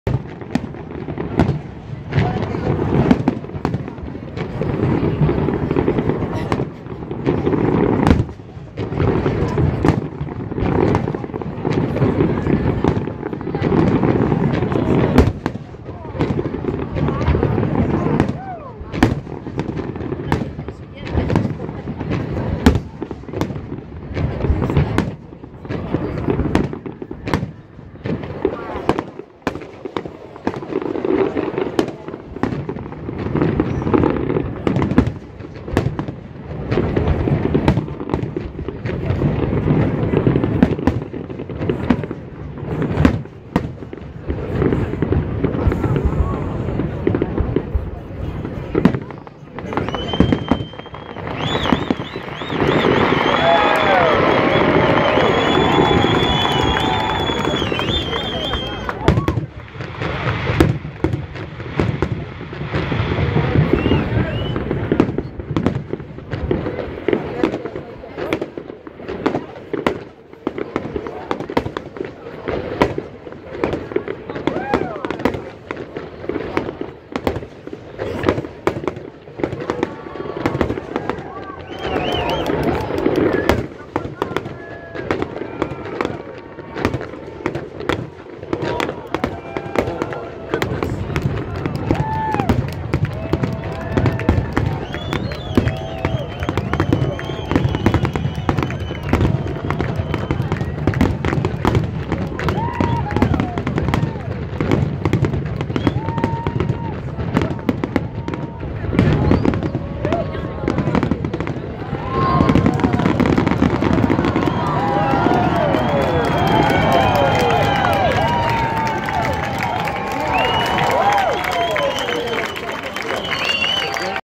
NYE Melbourne 2017 Fireworks | Final 2 minutes .. Amazing !
Fireworks filled the sky as Melbourne welcomed in 2017 ...

In Melbourne the skies lit up in the city’s greatest ever fireworks display. The city skyline was crowned with a golden sparkle as explosives fired from an unprecedented 22 rooftops.

Almost half a million revellers crammed into the CBD to view the glittering $500,000 spectacular.